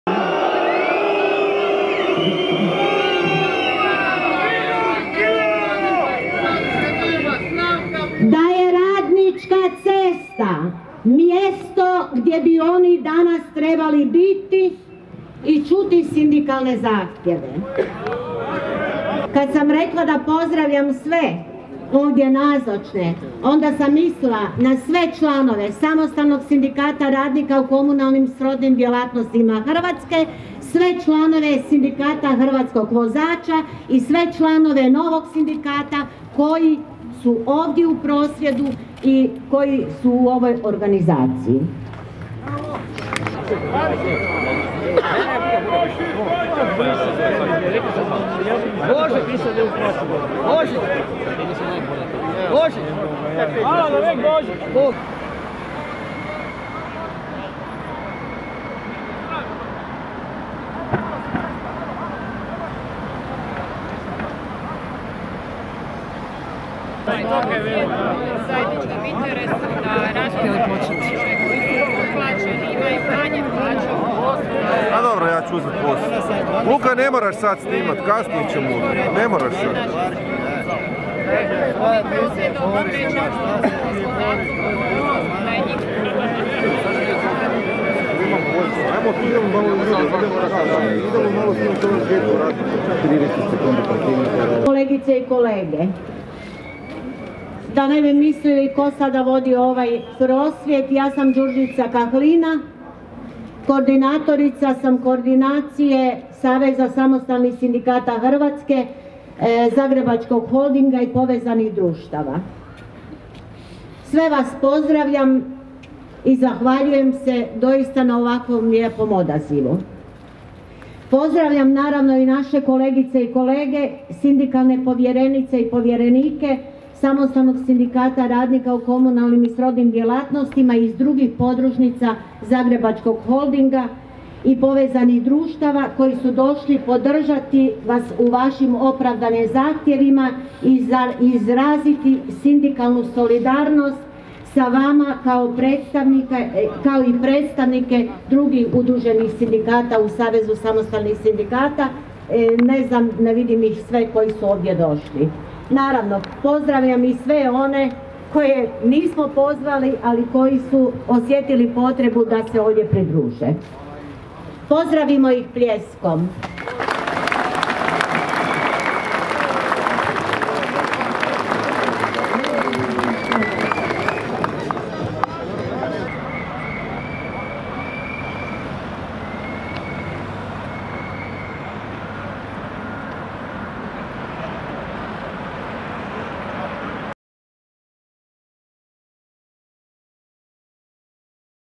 Prosvjed radnika Čistoće koji su nezadovoljni svojim primanjima
Ispred ulaza u zagrebačku gradsku tvrtku Čistoća organiziran je prosvjed  radnika nezadovoljnih uvjetima rada i visinom primanja.